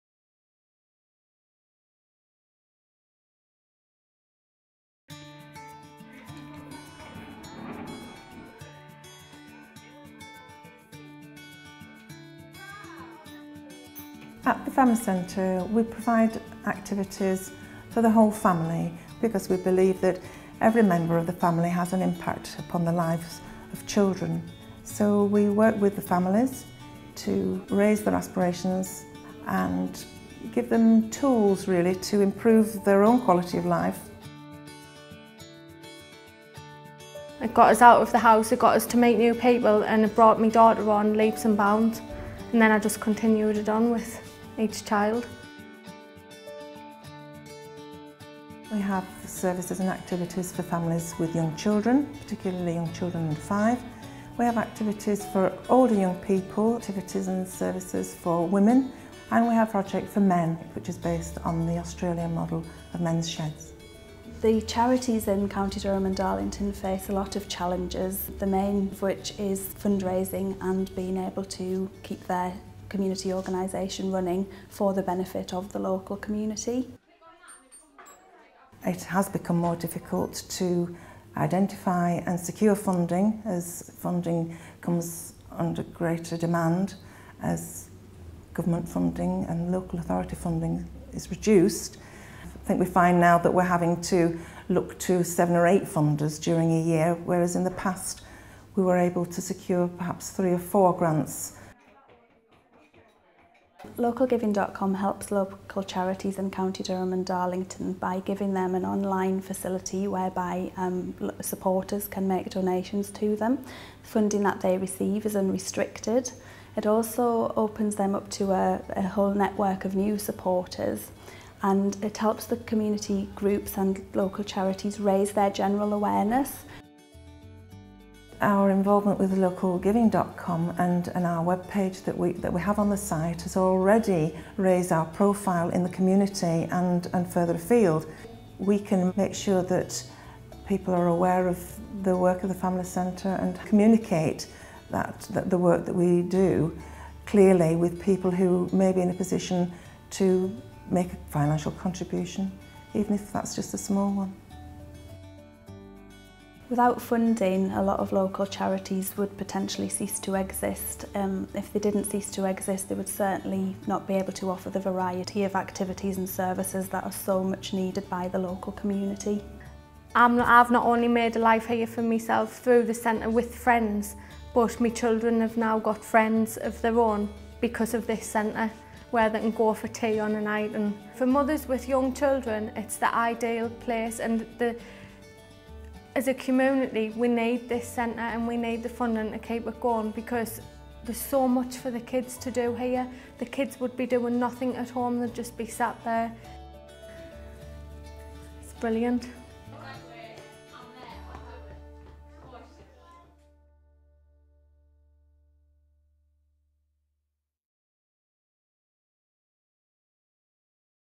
CDCF Localgiving.com - Wingate and Station Town Family Centre
Find out about Localgiving.com and the work of Wingate and Station Town Family Centre